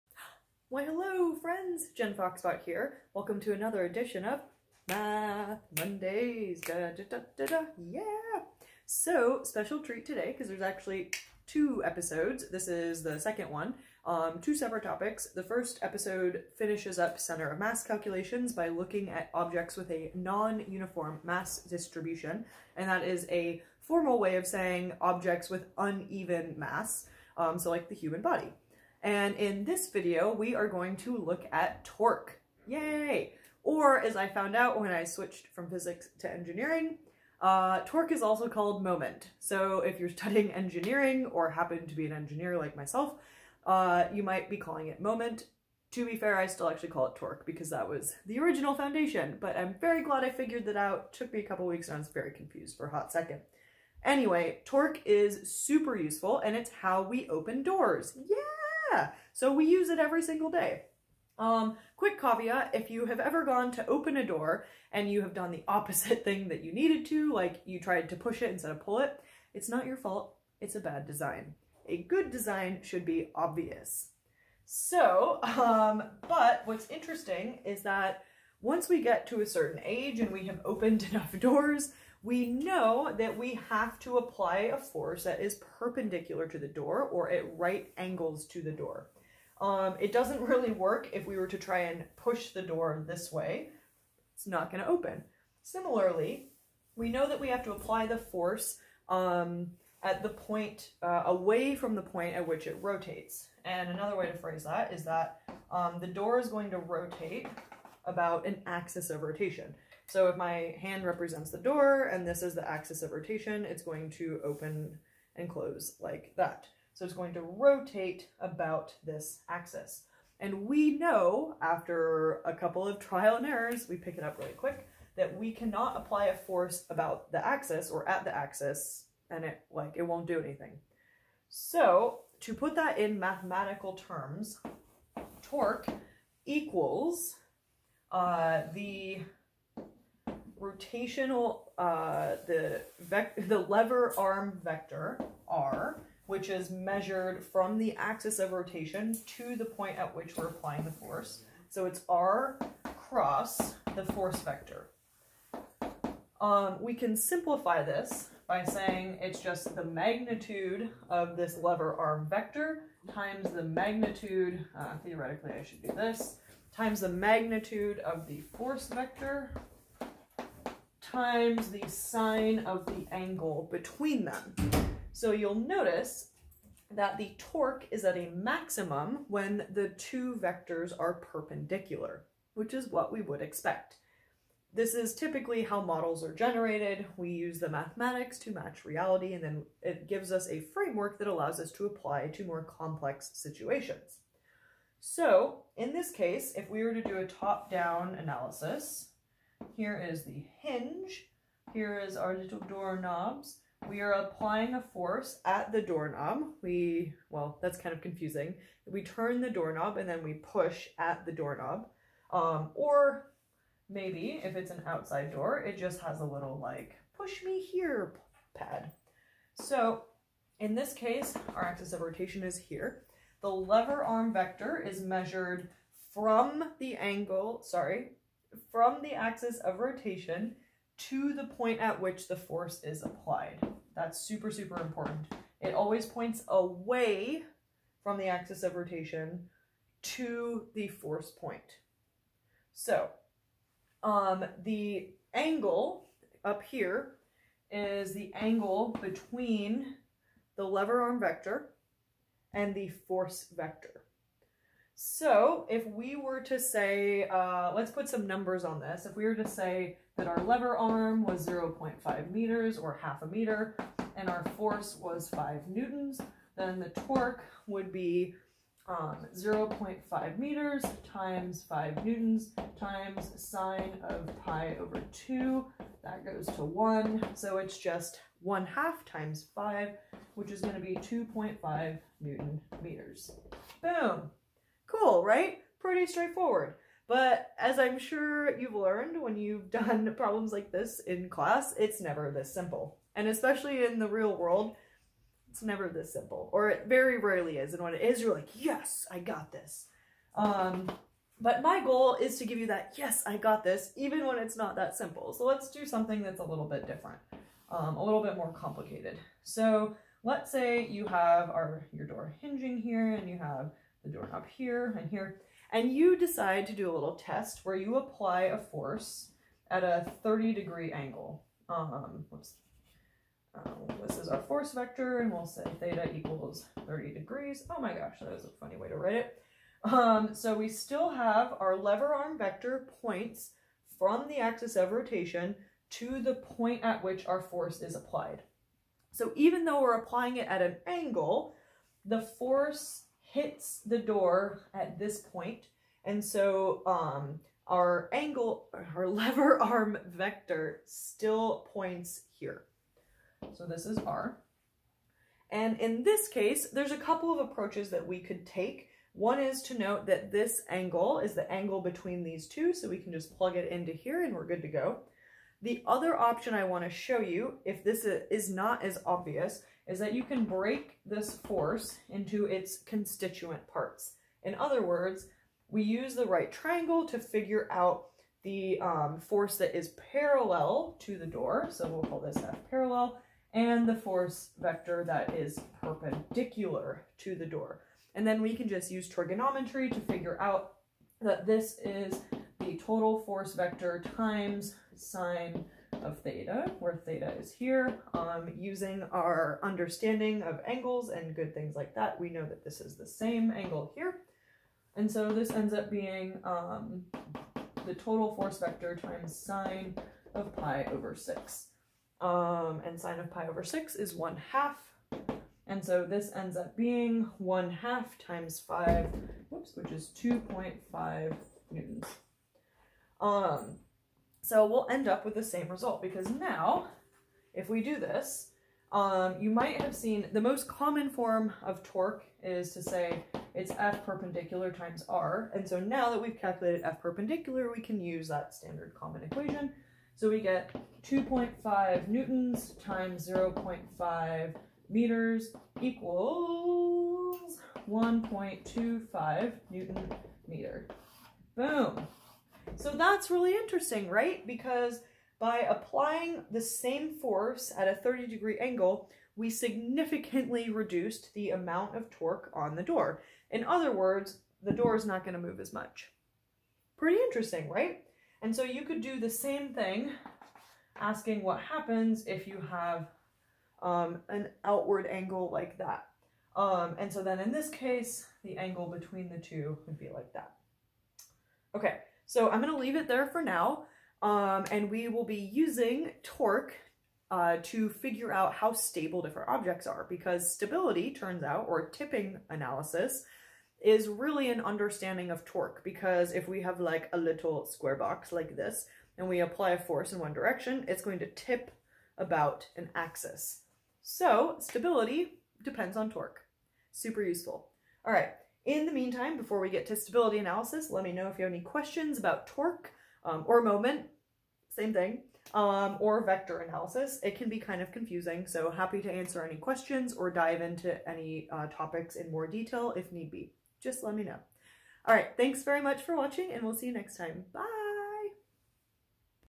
Torque ! (or moment (: )
Torque! The bane of every early physics student. Or moment, if you're in engineering :)

Torque is a twisting force that tends to cause rotation. AKA it's the force we use to open doors! Torque depends on the applied force as well as the distance from the axis of rotation, called the lever arm.

In this video, we use the door-opening example to figure out how to calculate torque for a "classic textbook problem" where the applied force is perpendicular, then do a trickier problem where the force is at a non-right-angle!